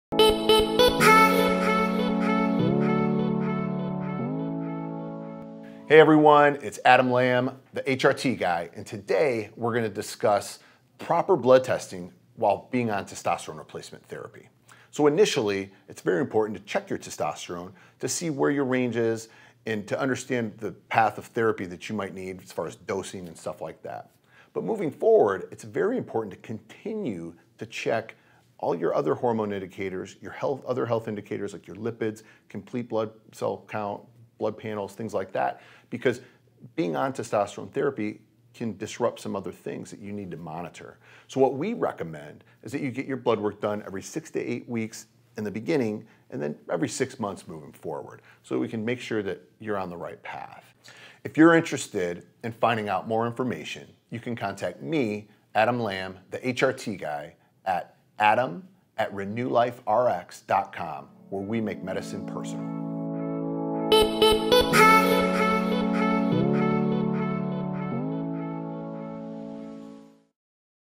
Proper blood work testing for testosterone replacement therapy
In this video we discuss what you should have tested when exploring your hormones for men and women. How often you should have your blood work done. What does it mean if you are low, high or in range?